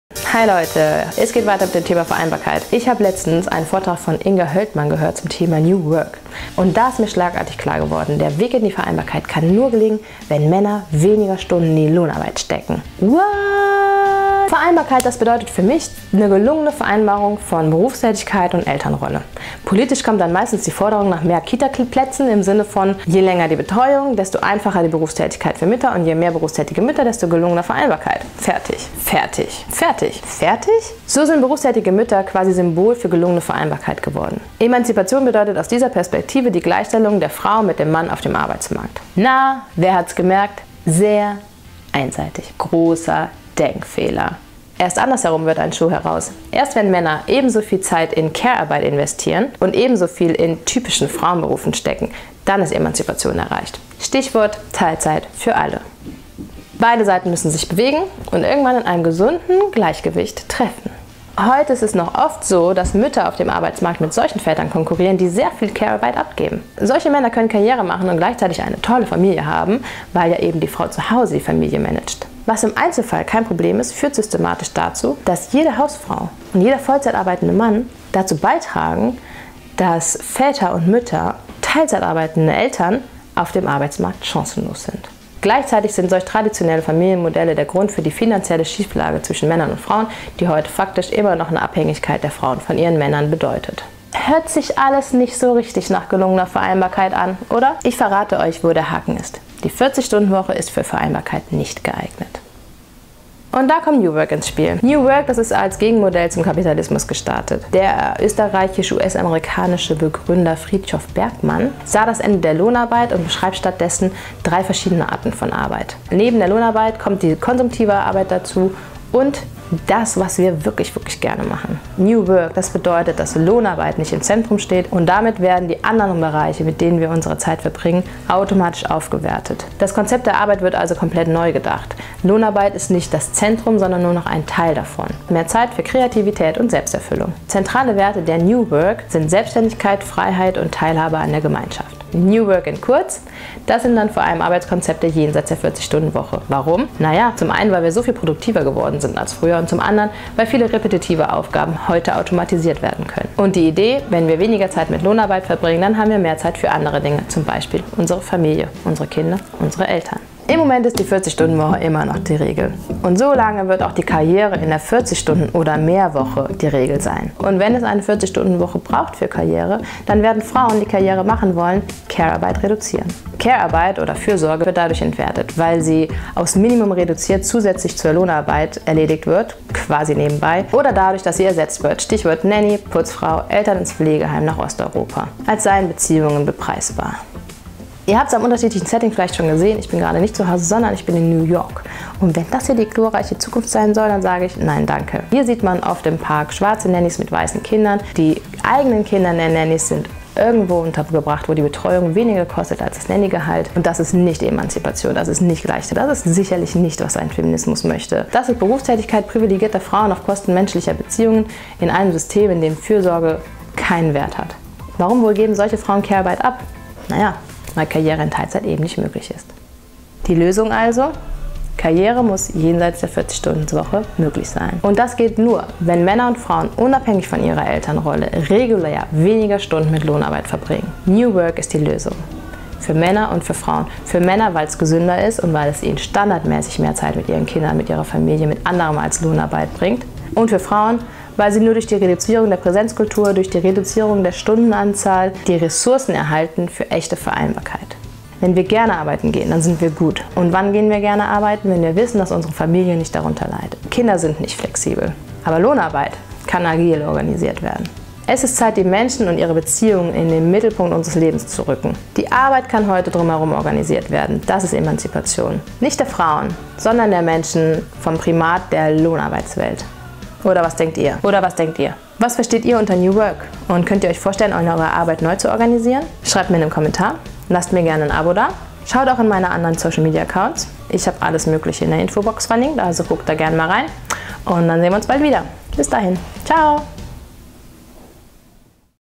Ist New Work die Lösung für Vereinbarkeit? | Gleichstellung durch weniger Stunden Lohnarbeit
Vereinbarkeit, das bedeutet für mich eine gelungene Vereinbarung von Elternrolle und Berufstätigkeit. Politisch kommt dann meistens die Forderung nach mehr Kitaplätzen. Da wird die einfache Korrelation aufgemacht: Je länger die Betreuung, desto einfacher die Berufstätigkeit für die Mütter und je mehr berufstätige Mütter, desto gelungener die Vereinbarkeit. Fertig. So sind berufstätige Mütter quasi Symbol für gelungene Vereinbarkeit geworden. Emanzipation bedeutet aus dieser Perspektive die Gleichstellung der Frau mit dem Mann auf dem Arbeitsmarkt.
Das halte ich für sehr verkürzt. Es war eine falsche Annahme, dass Emanzipation dadurch gelänge, dass Frauen ähnlich berufstätig sind wie Männer. Erst andersherum würde ein Schuh draus: Wenn Männer ebenso viel Carearbeit übernehmen und ebenso viel in „typischen Frauenberufen“ stecken, ist die Emanzipation erreicht. (Stichwort: Teilzeit für alle!)
Heute ist es oft so, dass Mütter auf dem Arbeitsmarkt mit Vätern konkurrieren, die viel Carearbeit abgeben. Solche Männer können erfolgreich sein und trotzdem eine „schöne Familie“ haben – die wird nämlich von der Frau gemanaged. Was im Einzelfall kein Problem ist führt systematisch dazu, dass jede Hausfrau und jeder vollzeitarbeitende Mann dazu beitragen, dass teilzeitarbeitende Mütter auf dem Arbeitsmarkt chancenlos sind. Gleichzeitig sind solch traditionelle Familienmodelle der Grund für die finanzielle Schieflage zwischen Männern und Frauen, die derzeit faktisch immer noch eine klare Abhängigkeit der Frauen von ihren Männern beschreibt.
Hört sich alles nicht so wirklich nach gelungener Vereinbarkeit an, oder? Ich verrate euch, wo der Haken ist: Die 40 Stunden Woche ist für Vereinbarkeit nicht geeignet.
Und da kommt New Work ins Spiel!
New Work, das ist als Gegenmodell zum Kapitalismus gestartet. Der österreichisch-US-amerikanische Begründer Frithjof Bergmann sah das Ende der „herkömmlichen LOHN-arbeit“ und beschreibt stattdessen drei verschiedene Arten von Arbeit: Neben der Lohnarbeit steht die konsumtive Arbeit und das, was wir wirklich gerne machen. Wenn die Lohnarbeit nur noch 1/3 Zeit beansprucht, bleibt mehr Zeit für Kreativität und Selbsterfüllung. Zentrale Werte der „New Work“ sind Selbstständigkeit, Freiheit und Teilhabe an der Gemeinschaft.
New Work in kurz: Das sind dann vor allem Arbeitskonzepte jenseits der 40 Stunden Woche. Warum? Naja, zum einen, weil wir heute so viel produktiver sind als früher und zum anderen, weil viele repetitive Aufgaben automatisiert werden können. Und die Idee: Wenn wir weniger Zeit mit Lohnarbeit verbringen, haben wir mehr Zeit für andere Dinge – zum Beispiel unsere Familie! Unsere Kinder, unsere Eltern.
Im Moment ist die 40 Stunden Woche noch die Regel. Und solange wird auch Karriere nur in der 40 Stunden (oder mehr!) Woche möglich sein. Und wenn es eine 40 Stunden Woche für Karriere braucht, werden Frauen, die Karriere machen wollen, Carearbeit reduzieren. Carearbeit wird entwertet, weil sie aufs Minimum reduziert zusätzlich zur Erwerbsarbeit geleistet wird – quasi nebenbei – oder dadurch, dass sie ersetzt wird – Stichwort Nanny, Putzfrau, eigene Eltern ins Pflegeheim nach Osteuropa. als seien Beziehungen bepreisbar.
Ich bin grade in New York – und wenn das die glorreiche Zukunft sein soll? Dann sage ich: Nein danke! Hier sieht man in Parks schwarze Nannys mit weißen Kindern. Die eigenen Kinder der schwarzen Nannys sind irgendwo betreut, wo die Betreuung weniger kostet als das Nannygehalt. Das ist nicht Emanzipation. Das ist Berufstätigkeit privilegierter Frauen auf Kosten menschlicher Beziehungen. Warum wohl geben diese Frauen Carearbeit ab? Na, weil Karriere in Teilzeit nicht möglich scheint.
Die Lösung also? Karriere muss jenseits der 40 Stunden Woche möglich sein. Und das geht nur, wenn Männer und Frauen, unabhängig von Elternschaft, regulär weniger Stunden mit Lohnarbeit verbringen.
New Work ist die Lösung. Für Frauen – und für Männer!
Für Männer, weil es gesünder ist und weil sie so standardmäßig Zeit mit ihren Kindern verbringen.
Für Frauen, weil sie nur durch die Reduzierung der Präsenzkultur und durch die Reduzierung der Stunden die Zeit und Ressourcen für echte Vereinbarkeit erhalten.
Es ist Zeit, die Menschen und ihre Beziehungen in den Mittelpunkt unseres Lebens zu rücken. Die Arbeit kann heute drumherum organisiert werden. Das ist Emanzipation. Nicht der Frauen, sondern der Menschen; vom Primat der Arbeitswelt.
Oder was denkt ihr? Was versteht ihr unter New Work und könnt ihr euch vorstellen, eure Arbeit neu zu organisieren? Schreibt mir nen Kommentar, ich bin gespannt auf eure Meinung!